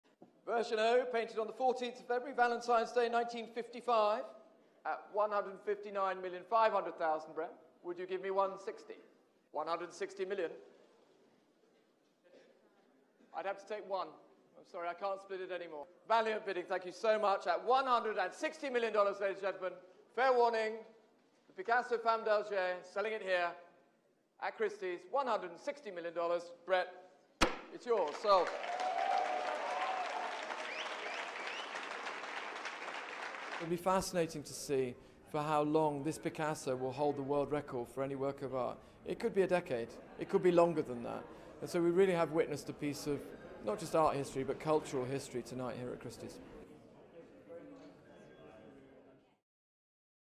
Pablo Picasso's Women of Algiers sets new world record at New York auction
Pablo Picasso's oil painting, Women of Algiers (Version O) has set a new world record at auction, selling for $179.4m (£115.2m) in New York.
Women of Algiers (Version O) is part of a series of 15 paintings created in 1954 and 1955 by Pablo Picasso. The successful bid was made via telephone by an anonymous buyer. Christie's auctioneer Jussi Pylkkänen says the sale was 'a piece of cultural history'.